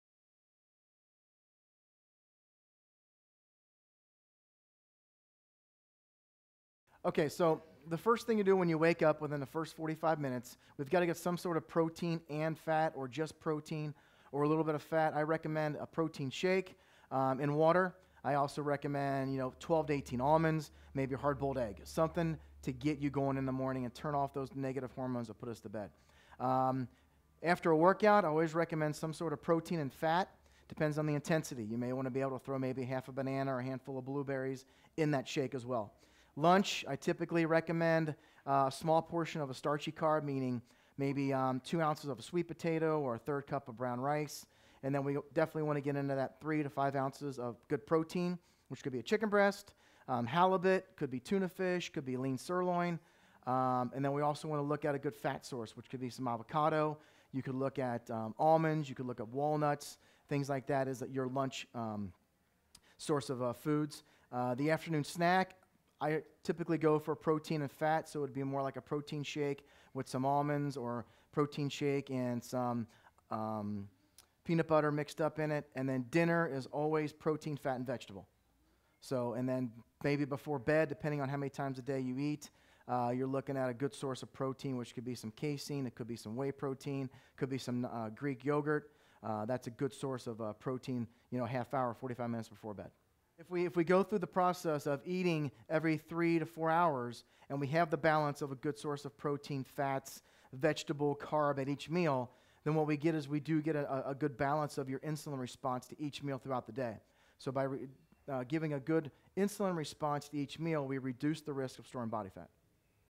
Chuck Rudolph - What Your Daily Meal Plan Should Look Like
Registered Dietitian, Chuck Rudolph, gives the BodyBare Fitness Bootcamp Team some advice on nutrition, diet and health for your life.